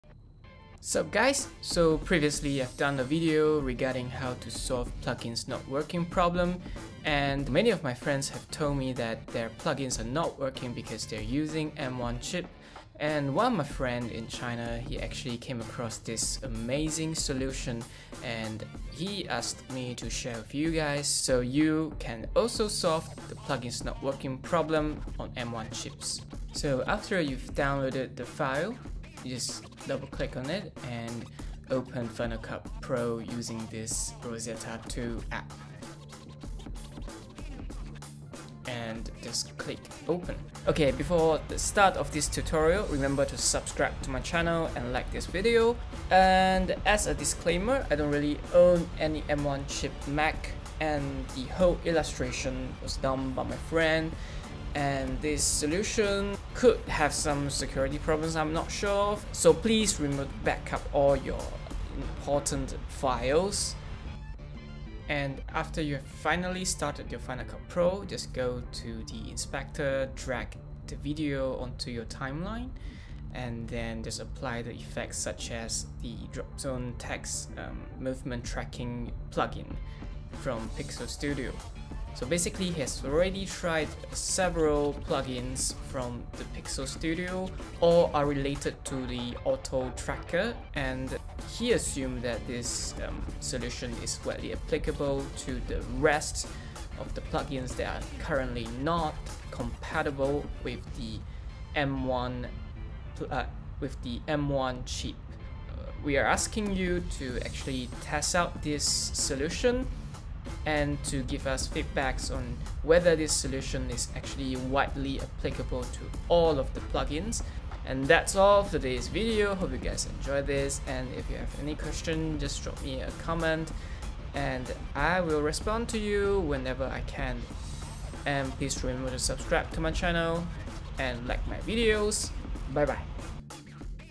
M1 Chip Mac Plugins Not Working Problem in FCPX (Possible Solutions)
Hi, if you are experiencing the annoying "plugins not working" problem in Final Cut Pro X (FCPX), and found no solutions because you are using an M1 chip mac, try this solution.